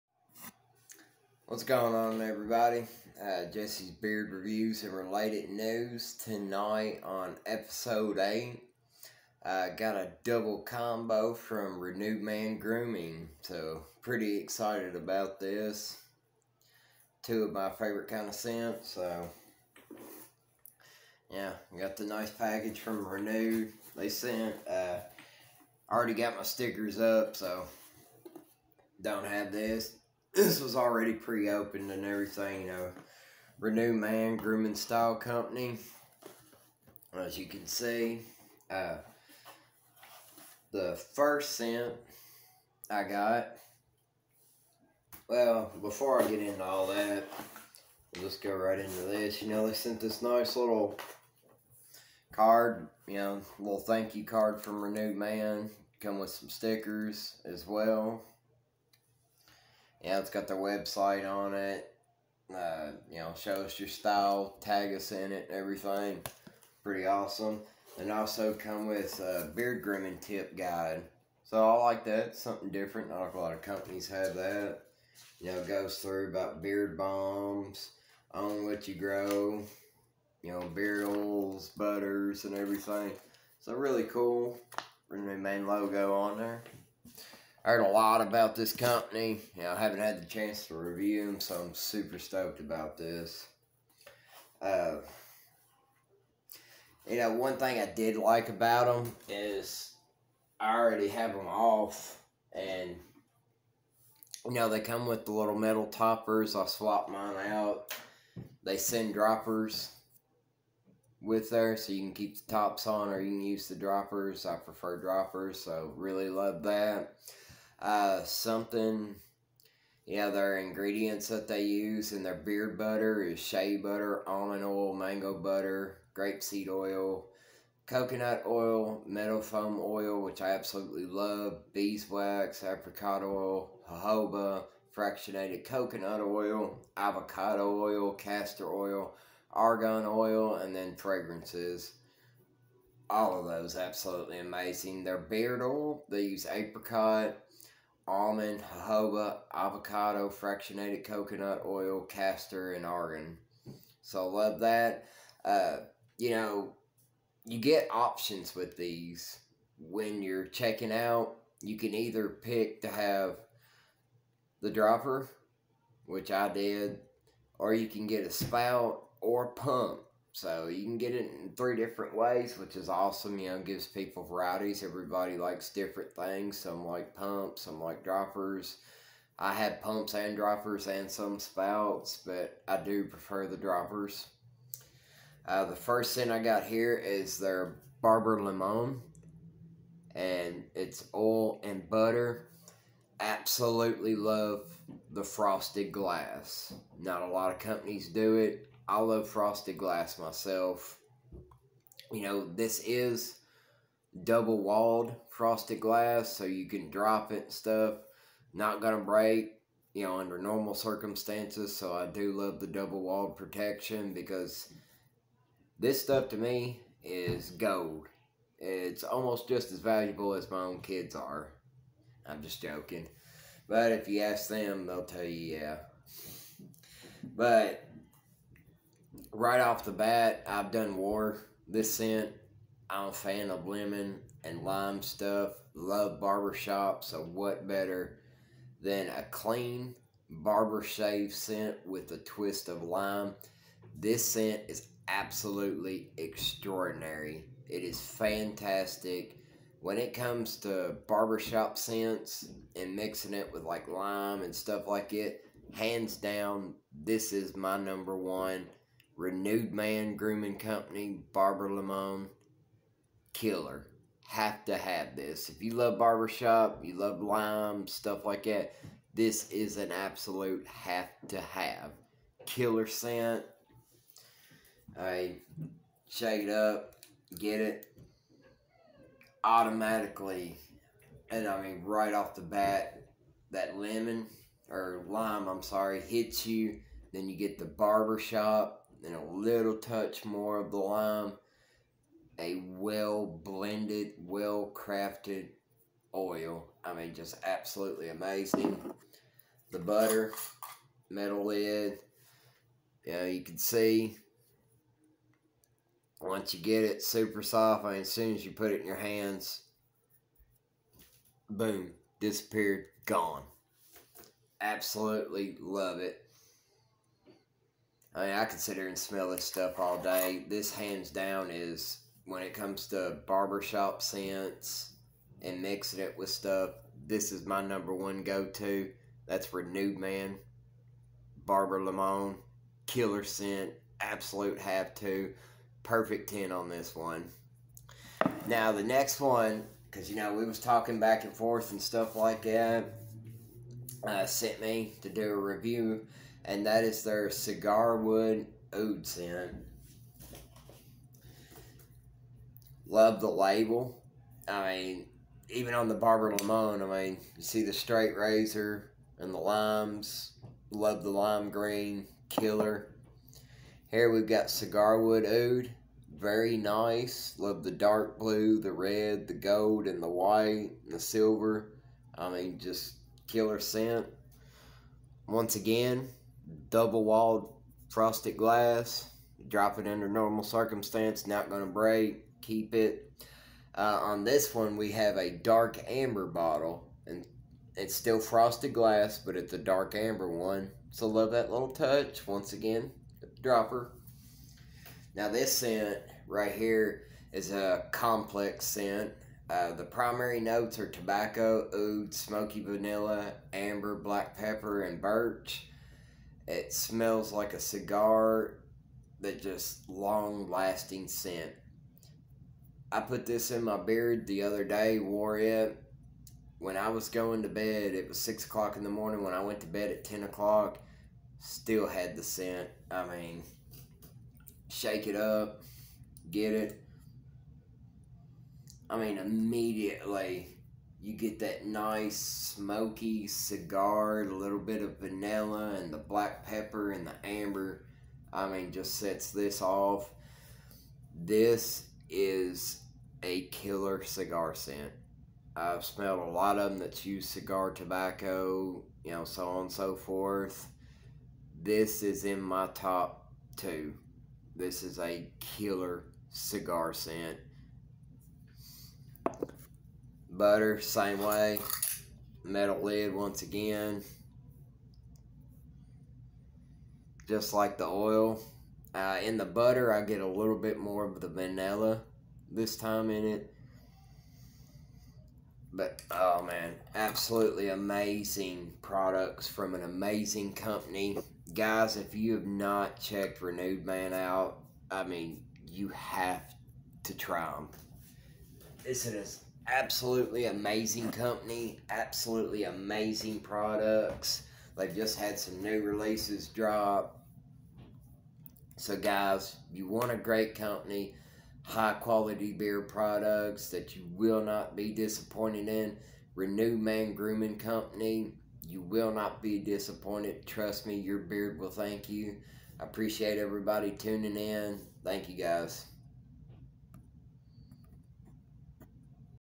episode 8 renewed man groom and style company two scent combo Review
review of renewed man groom and style co barber Limon oil and butter and cigarwood oud scents